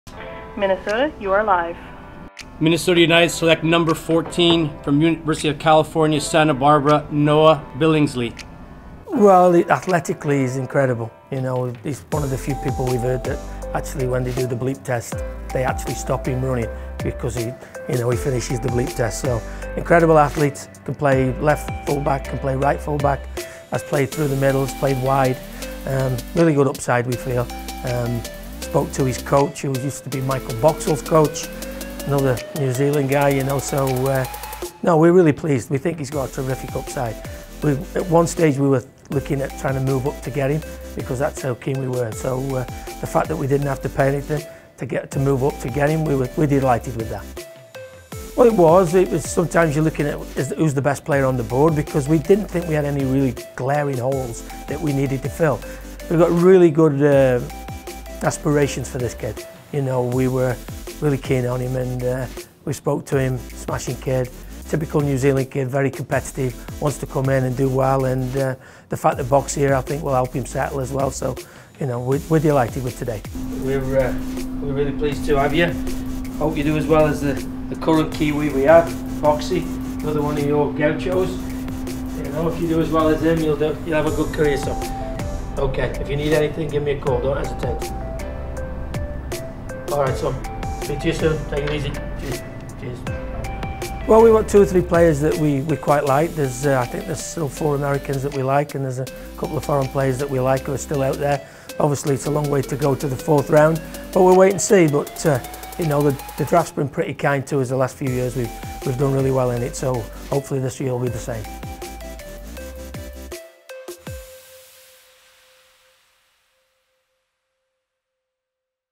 2020 MLS SuperDraft Day One
Head Coach Adrian Heath talks about 18th overall selection Noah Billingsley as the league wraps up the first two rounds of the 2020 MLS SuperDraft.